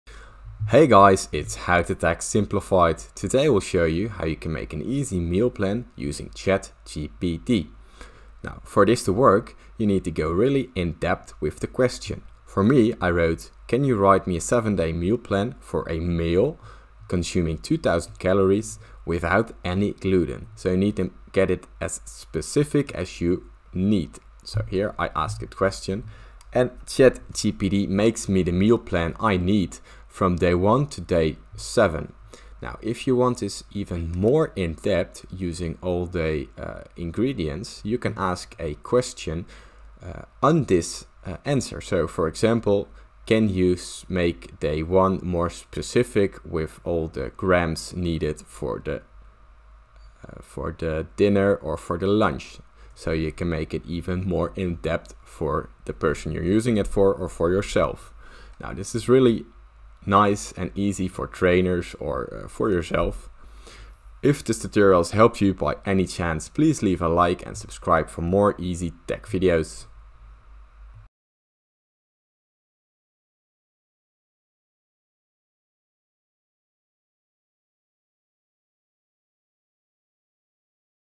How To Use Chat GPT To Write A Meal Plan
In this tutorial, we'll show you how to use OpenAI's GPT-3 language model to create a custom meal plan tailored to your specific dietary needs and preferences. We'll walk you through the process of inputting information such as your goals, dietary restrictions, and favorite foods, and show you how GPT-3 generates a personalized meal plan in response.

1.    Input the relevant information: Once Chat GPT is set up, you can input the information about your nutritional needs and preferences into the model. This may include things like your age, weight, height, activity level, and any dietary restrictions or goals.
2.    Request a meal plan: To request a custom meal plan from Chat GPT, you can use a specific command or prompt, such as "Please create a meal plan for me" or "I need a meal plan that meets the following criteria: (insert criteria here)."
3.    Review the meal plan: Chat GPT will generate a custom meal plan based on the information you input. Review the meal plan to ensure that it meets your nutritional needs and preferences, and make any necessary adjustments.
4.    Implement the meal plan: Once you're satisfied with the meal plan generated by Chat GPT, you can start implementing it in your daily routine. Be sure to track your progress and make any necessary adjustments to the plan as needed.

Keep in mind that the quality of the meal plan generated by Chat GPT will depend on the accuracy of the information you input and the effectiveness of the model. It may take some trial and error to produce a meal plan that meets your nutritional needs and preferences.